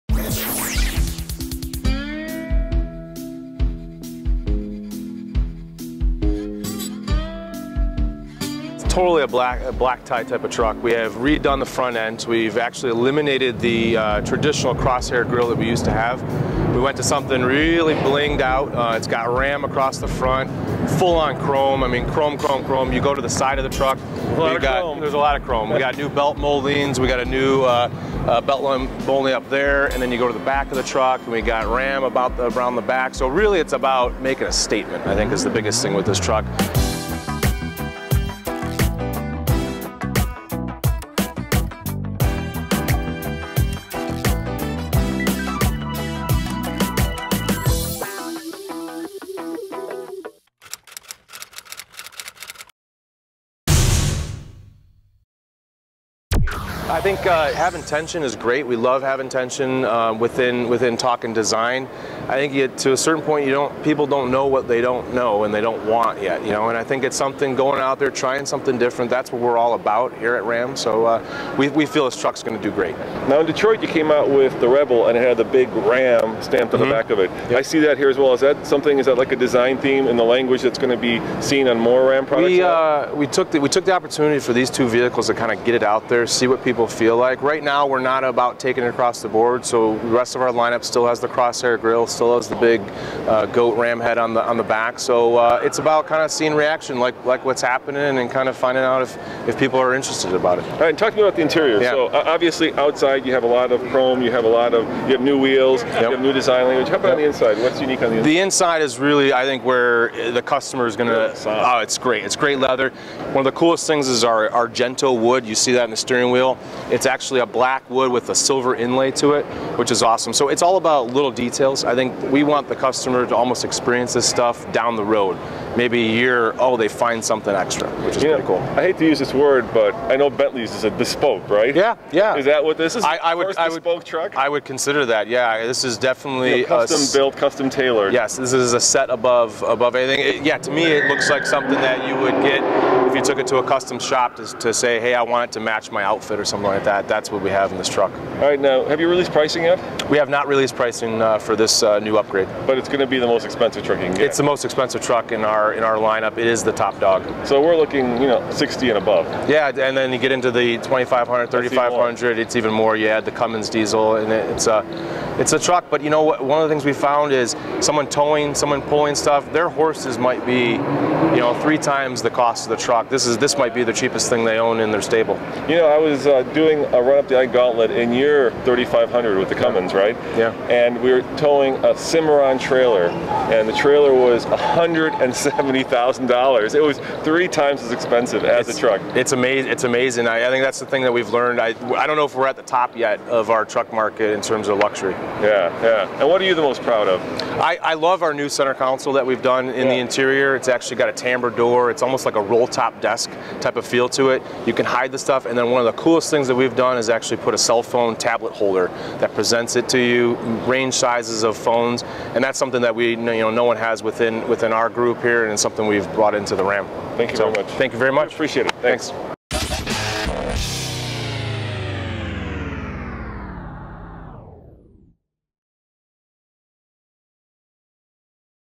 2015 Ram 1500 Limited: What You Need To Know
) The 2015 Ram 1500 Limited is the new top dog in the Ram pickup truck stable. Yes, the Laramie Limited grade has been around before now, but the company is taking this luxury truck to the next level with new exterior touches and interior materials. Gone is the "cross-hair" grille. There is a lot of chrome on the outside with the big "RAM" spelled out in the front and in the rear. Interior is all black with new leather and wood to make for a luxurious atmosphere.